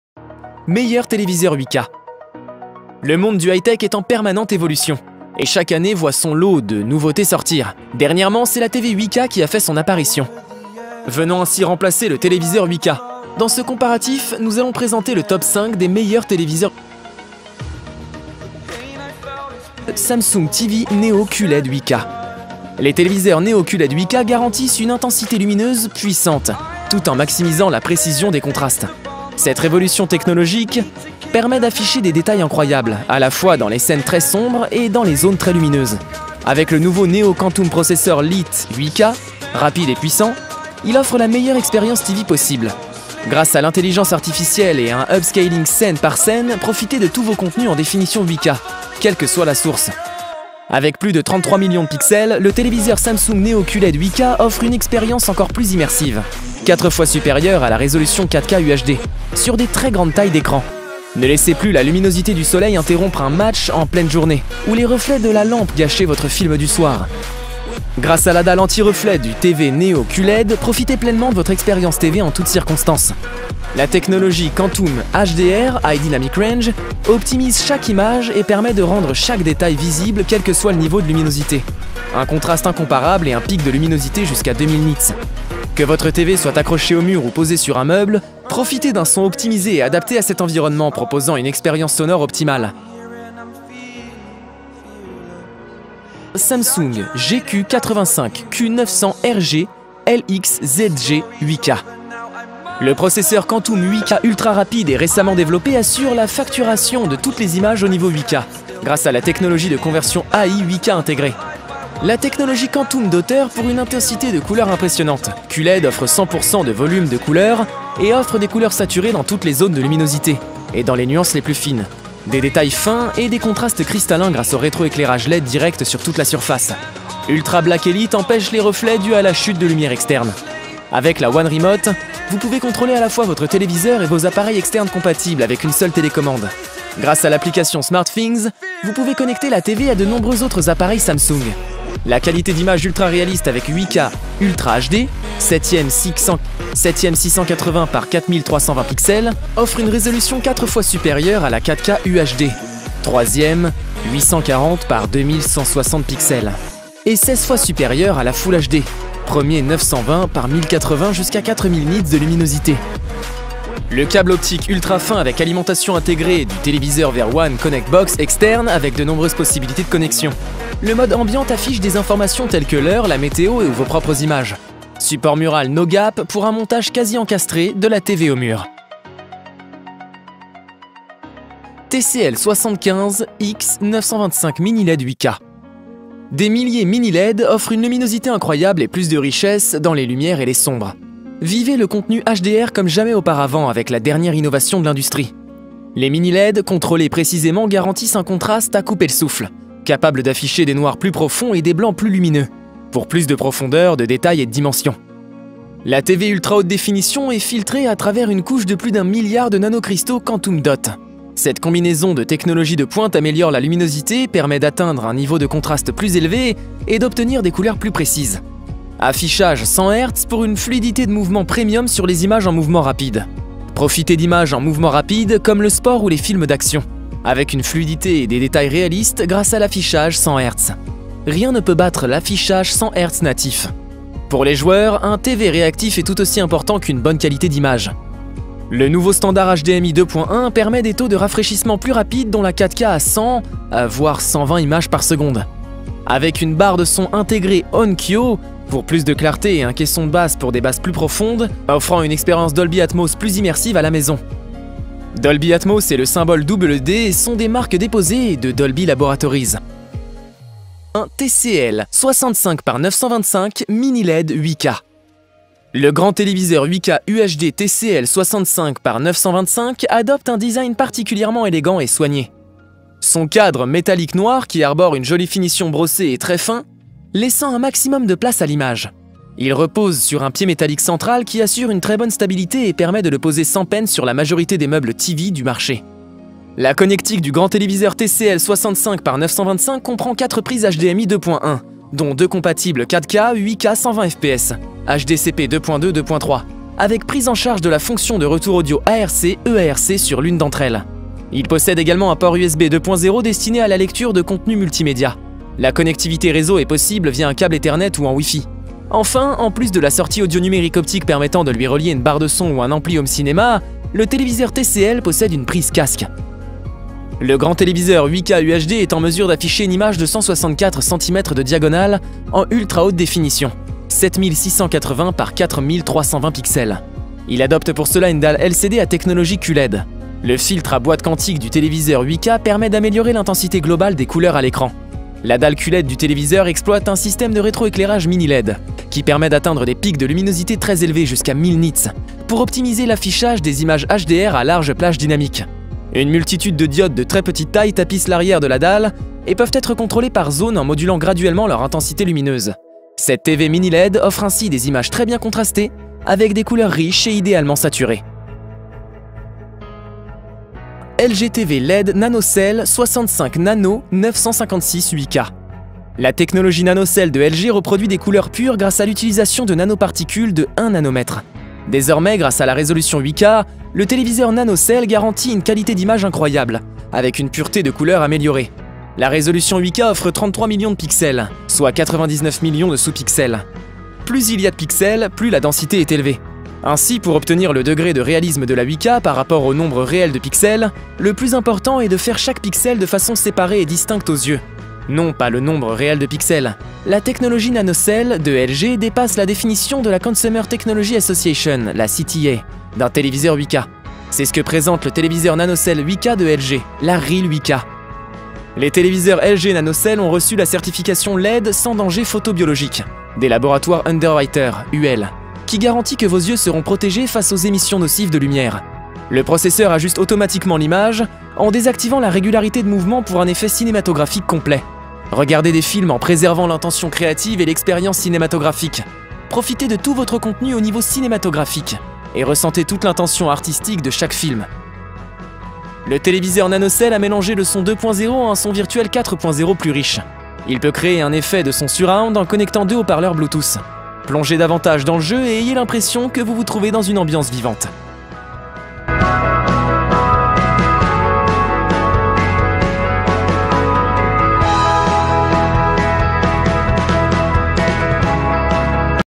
TOP 5 : Meilleure TV 8K 2025 Quel Téléviseur choisir ? Rapport Qualité / Prix
Le monde du high-tech est en permanente évolution, et chaque année voit son lot de nouveautés sortir. Dernièrement, c’est la TV 8K qui a fait son apparition, venant ainsi remplacer le téléviseur 4K, dans ce compratif nous allons présenter le top 5 des meileurs téléviseurs 8K.

Liens D'achat :
1- samsung tv neo qled 8k
2- Samsung GQ85Q900RGLXZG 8K
3- TCL 75X925 Mini LED 8K
4- TCL 65X925 Mini LED 8K
5- LG TV LED NanoCell 65NANO956 8K